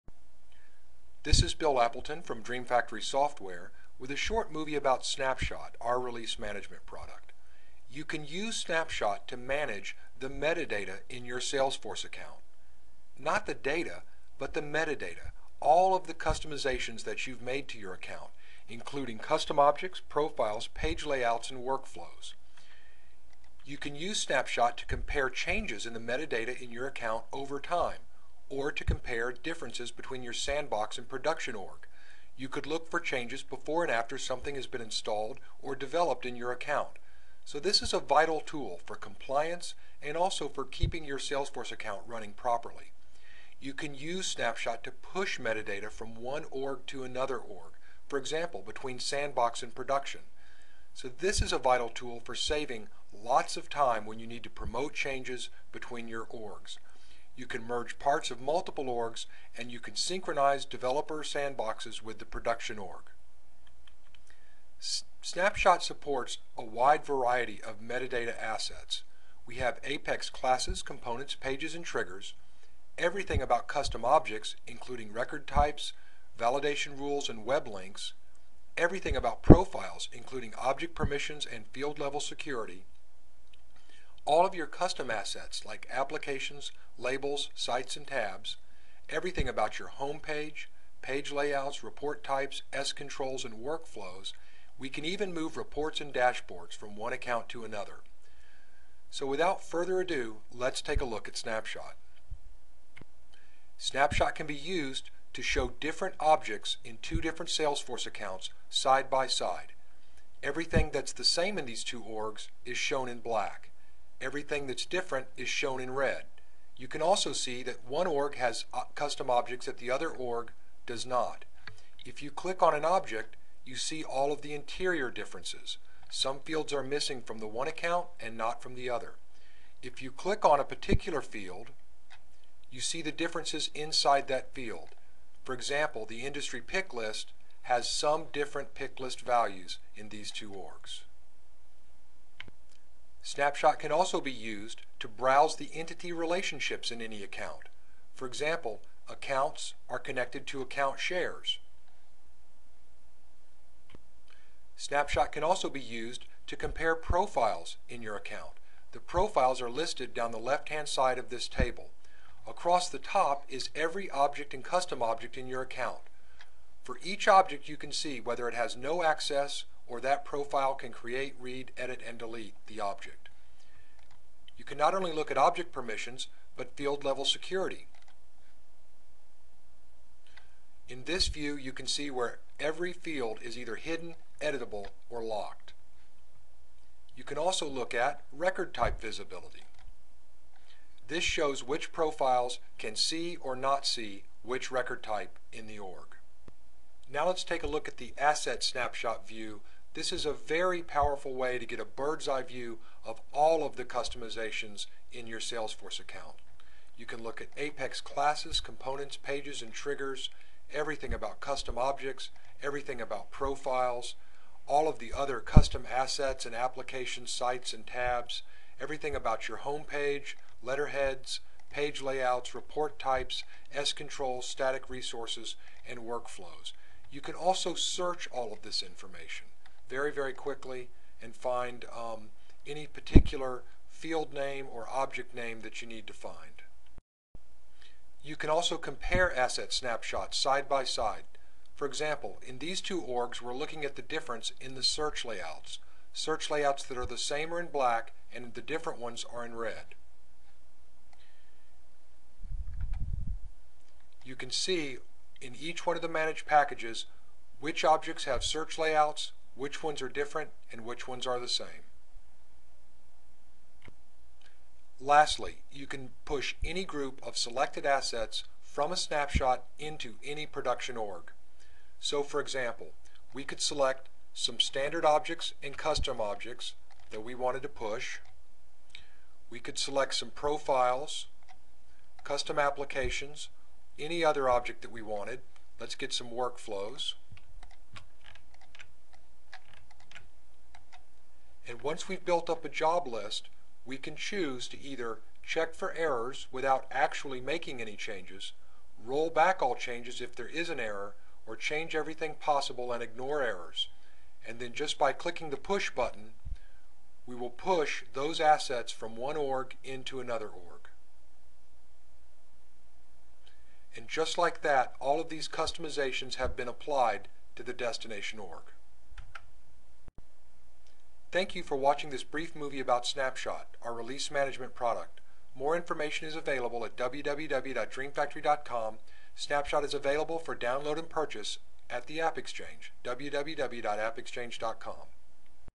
SnapShot Release Management
System Administrators can use SnapShot to manage, compare, and push customizations between any org, including sandbox and production. Metadata support includes custom objects, page layouts, profiles, workflows, reports, dashboards, scontrols, and more!